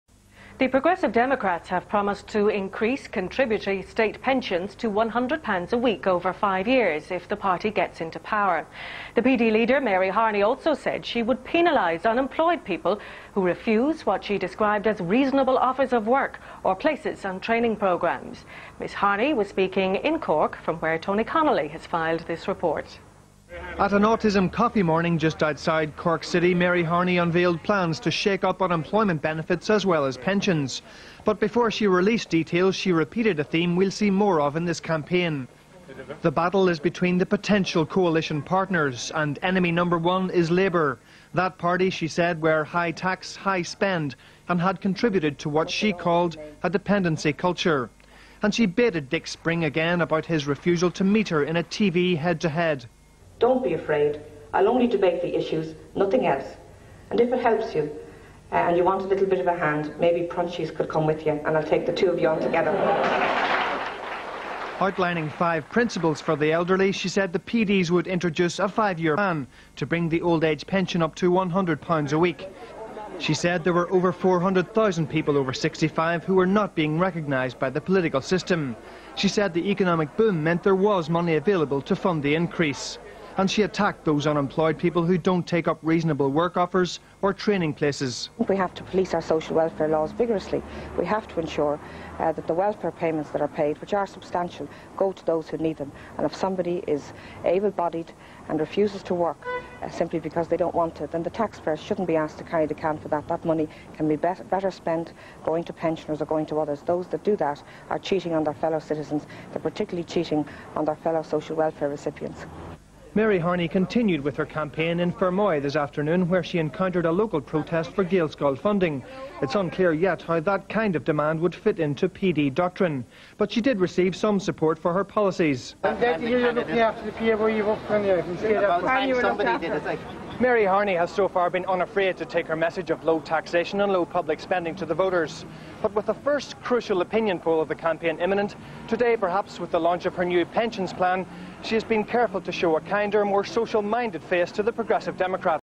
Progressive Democrats launch Election 97 manifesto
On the 20th of May 1997 P.D Leader Mary Harney launched her party's manifesto citing Labour as it's main adversary and repeating her challenge to Tánaiste Dick Spring for a televised debate